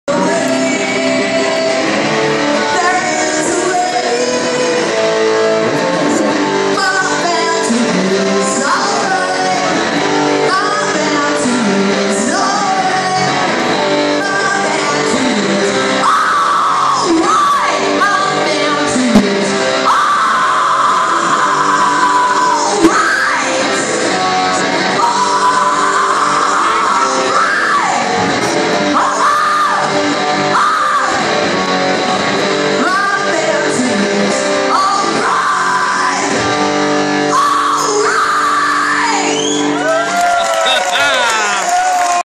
Kim shattuck is cool.
Snip of pixies cover. Coach house 3/22/13